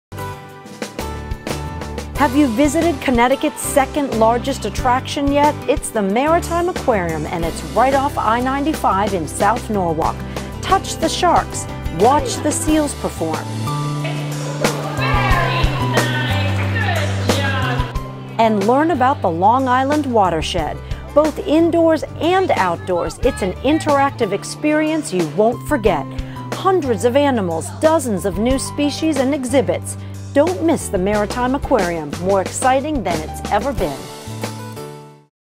Tourism Kiosk
Did you know? The Maritime Aquarium is Connecticut's second-largest attraction! Learn about Long Island Sound and get close to our animals, including many interactive touch exhibits. Touch a moon jelly in our "Jiggle a Jelly" exhibit, pet a shark or a stingray at our Shark & Ray touch pool, and touch crabs, sea stars and more at our Intertidal Touch Tank. Come visit us, we're right off I-95 in Norwalk!